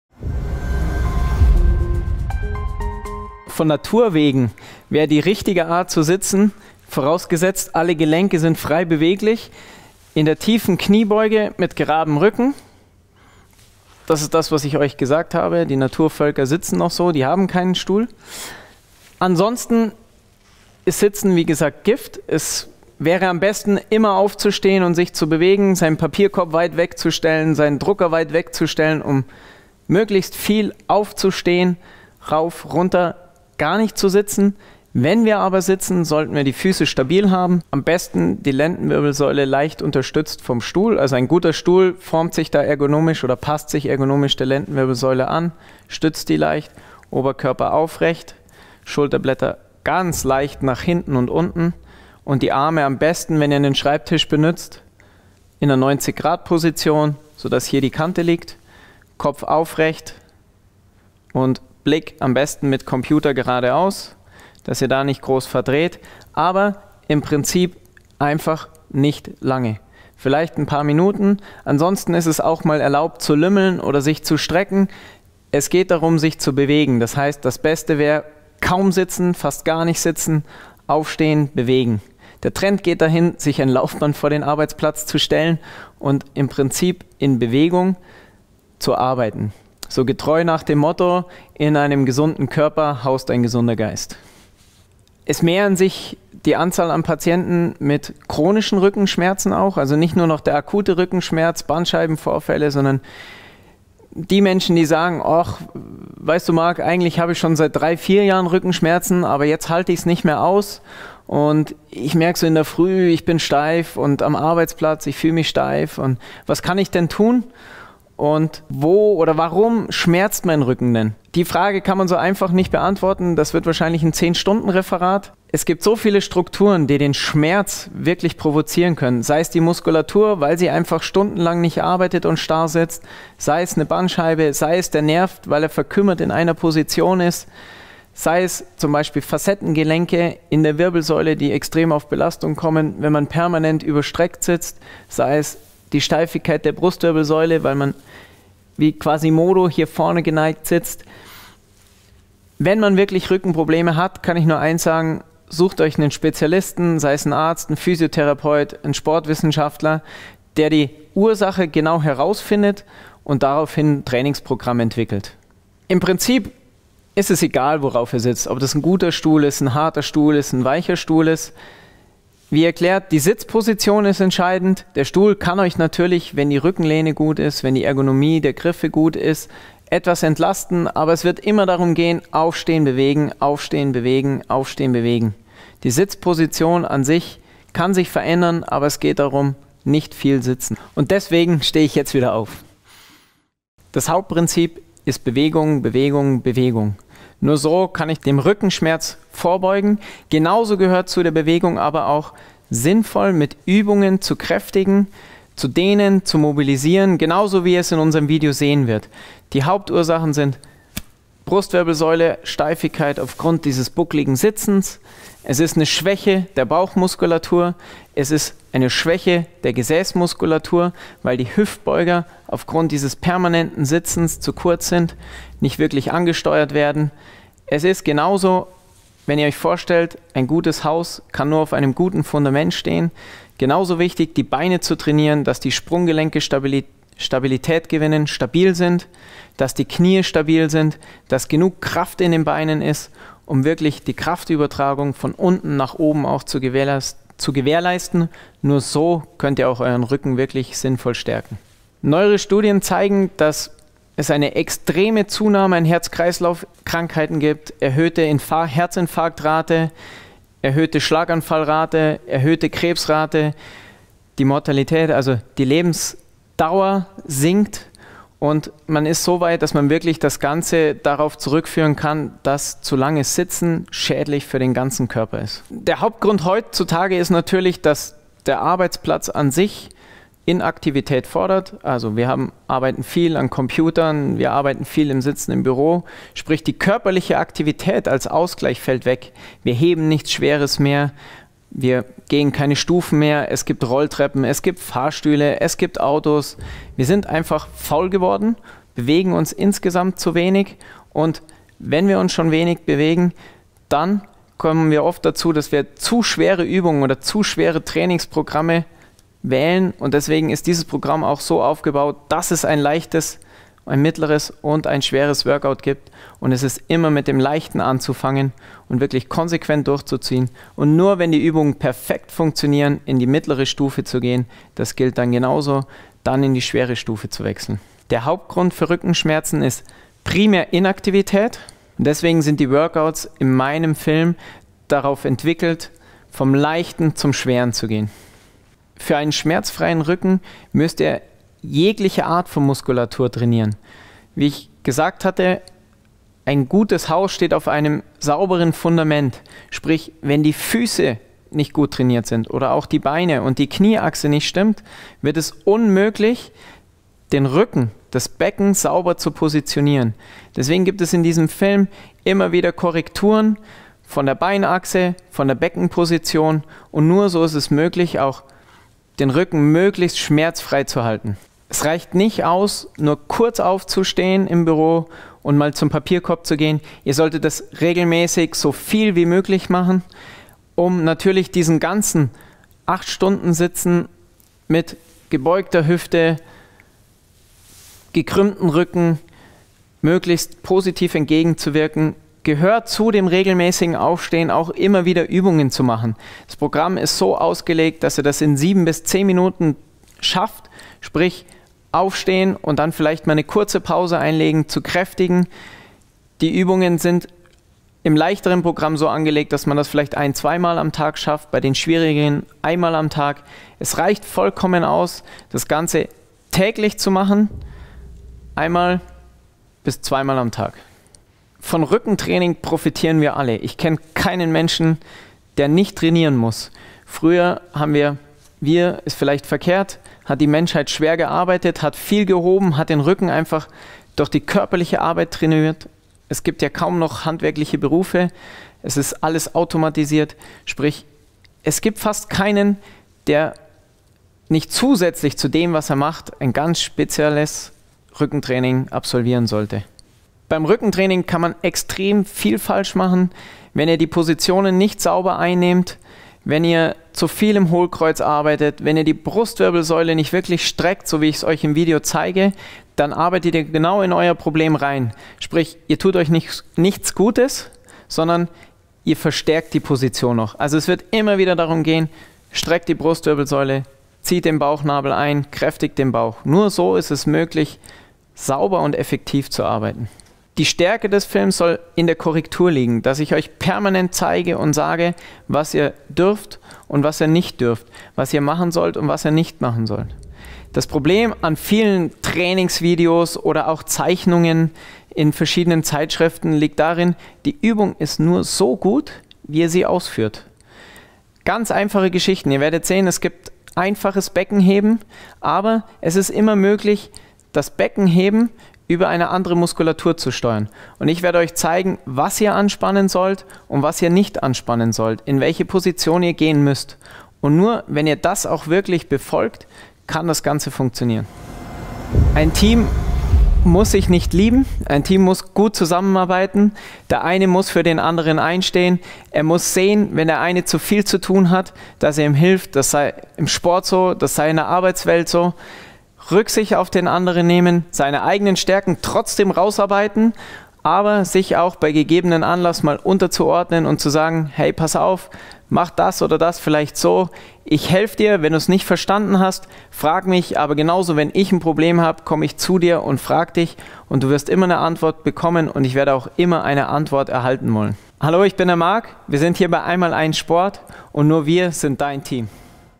Die Gefahren des Sitzens - Falsches Sitzen verstehen und vermeiden 💪🏼 Richtiges Sitzen lernen!
Die Gefahr des Sitzens - Wie dein Rücken im Büro gesund bleibt!

Einseitige Belastungen und Körperhaltungen sind schlecht für den Körper. Beim Sitzen wird in erster Linie der Rücken zum Leidtragenden. Allerdings kommen die meisten von uns nicht darum herum, den Arbeitstag im Büro (im Sitzen) zu verbringen. Wie Du trotzdem das Schlimmste abwenden kannst und den Büroalltag auch auf lange Sicht schmerzfrei überstehst, erfährst Du von unseren Rücken-Profis im Film.

Diplom-Sportwissenschaftler Marc Lechler betreut als Reha- und Athletiktrainer verschiedene Leistungssportler, Profis und Vereine. Außerdem hat er sich auf präventive Rückenvorsorge spezialisiert. Für diesen Film hat er seine effektivsten Übungen ausgepackt und zeigt Dir, wie Du mit einfachen Übungen für einen starken, gesunden und schmerzfreien Rücken sorgst. Unterstützt wird er dabei von Sportmediziner Dr. Christian Mathonia, dem Mannschaftsarzt des TSV 1860 München und verschiedener DFB-Jugend­national­mannschaften

Hat dir dieses Video gefallen ? - Dann schau doch in unseren Film DEIN GESUNDER & STARKER RÜCKEN rein